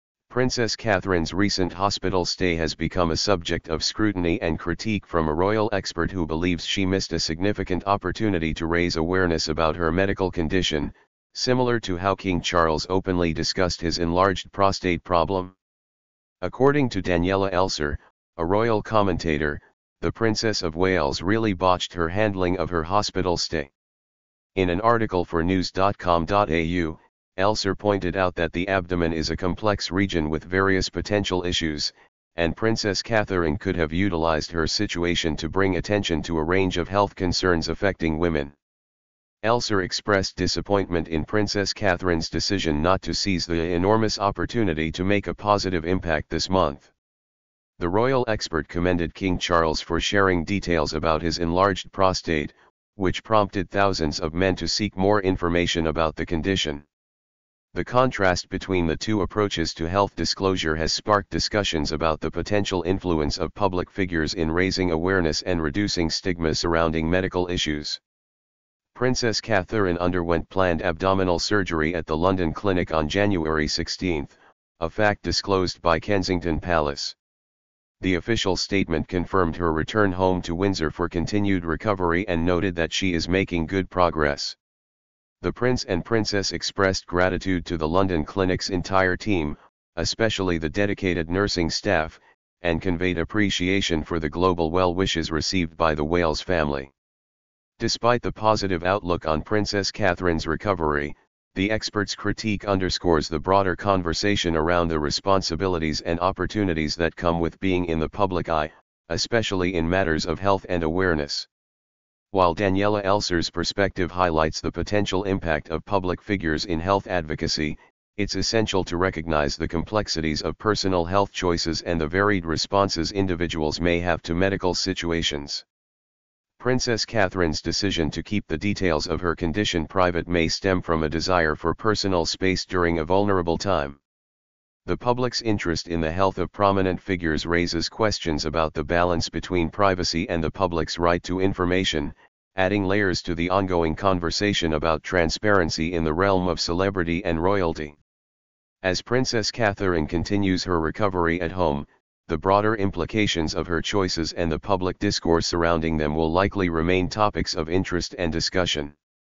Princess Kate misses 'huge opportunity' to make impact like King Charles
Princess Catherine's recent hospital stay has become a subject of scrutiny and critique from a royal expert who believes she missed a significant opportunity to raise awareness about her medical condition, similar to how King Charles openly discussed his enlarged prostate problem. According to Daniela Elser, a royal commentator, the Princess of Wales "really botched her handling of her hospital stay." In an article for news.com.au, Elser pointed out that the abdomen is a complex region with various potential issues, and Princess Catherine could have utilized her situation to bring attention to a range of health concerns affecting women.

Elser expressed disappointment in Princess Catherine's decision not to seize the "enormous opportunity" to make a positive impact this month. The royal expert commended King Charles for sharing details about his enlarged prostate, which prompted thousands of men to seek more information about the condition. The contrast between the two approaches to health disclosure has sparked discussions about the potential influence of public figures in raising awareness and reducing stigma surrounding medical issues.

Princess Catherine underwent planned abdominal surgery at The London Clinic on January 16, a fact disclosed by Kensington Palace. The official statement confirmed her return home to Windsor for continued recovery and noted that she is making good progress. The Prince and Princess expressed gratitude to The London Clinic's entire team, especially the dedicated nursing staff, and conveyed appreciation for the global well wishes received by the Wales family. Despite the positive outlook on Princess Catherine's recovery, the expert's critique underscores the broader conversation around the responsibilities and opportunities that come with being in the public eye, especially in matters of health and awareness.

While Daniela Elser's perspective highlights the potential impact of public figures in health advocacy, it's essential to recognize the complexities of personal health choices and the varied responses individuals may have to medical situations. Princess Catherine's decision to keep the details of her condition private may stem from a desire for personal space during a vulnerable time. The public's interest in the health of prominent figures raises questions about the balance between privacy and the public's right to information, adding layers to the ongoing conversation about transparency in the realm of celebrity and royalty. As Princess Catherine continues her recovery at home, the broader implications of her choices and the public discourse surrounding them will likely remain topics of interest and discussion.